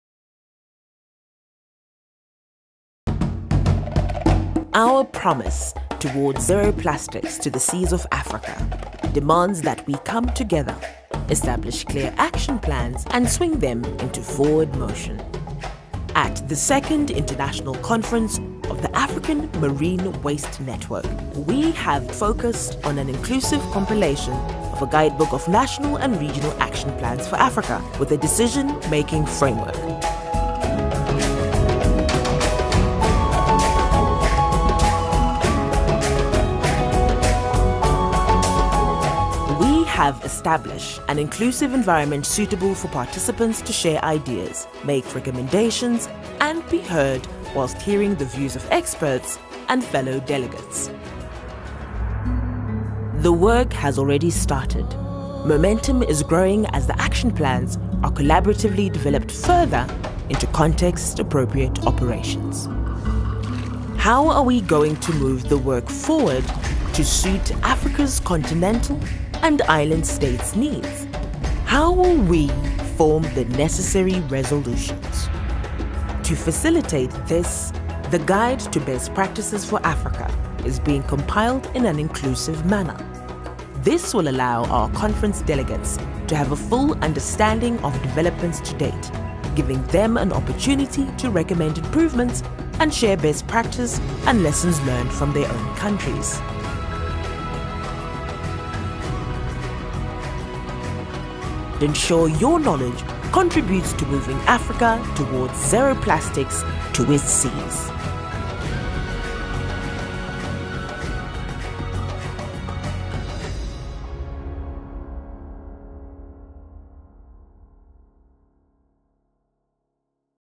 The Guidebook to Key Actions for Plastics Management in Africa
Sustainable Seas Trust, in collaboration with many passionate organisations and individuals across Africa, is drafting a Guidebook to Develop National and Regional Action Plans, with a Decision-Making Framework for Management of Plastics in Africa.

The Guidebook will reflect the voice of every organisation that attended our recent 'Zero Plastics to the Seas of Africa' conference and beyond. We invite you to send your case studies of best practices or design principles based on lessons learned to to include in the Guidebook.